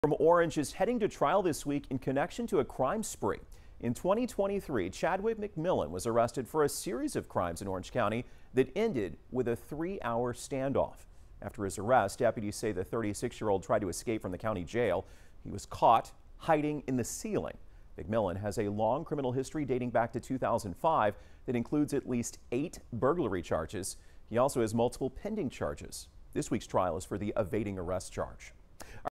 Orange man is heading to trial this week in connection to a 2023 crime spree
In 2023 McMillin went on a crime spree that ended in a police stand-off